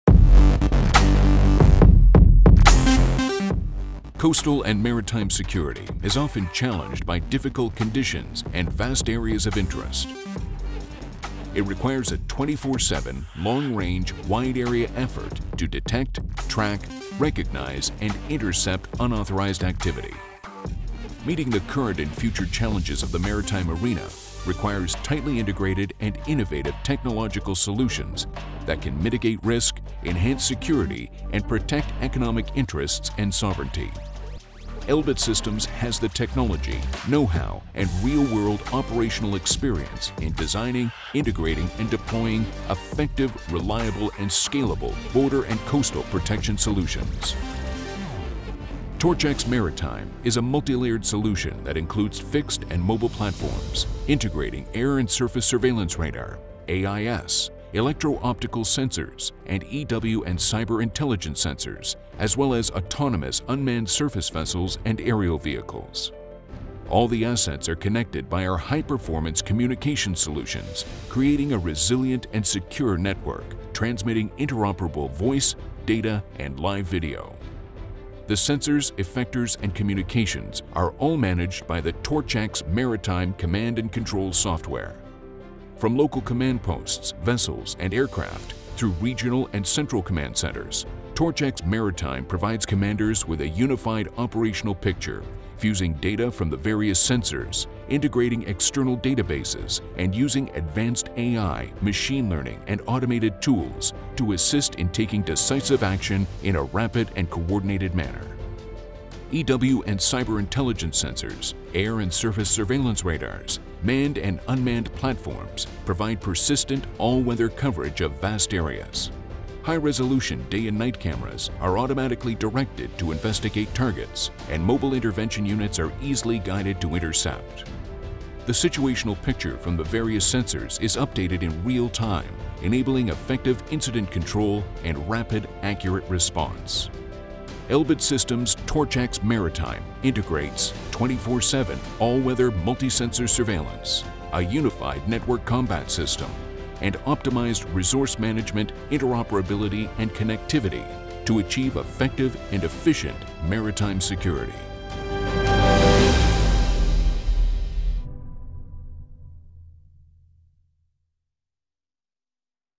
Elbit Systems / TORCH-X™ MARITIME
Torch-X Maritime is a multi-layered, integrated solution that combines various maritime sensors and tactical elements. The solution enables highly effective cross-force coordination, quick and precise planning and execution with effective sensor-to-responder capabilities, as well as simulation and training capabilities for mission planning and development of emergency procedures. Advanced AI tools assist the decision-making process by recommending tasks to the commanders, on all available data, and in accordance with the rules of engagement.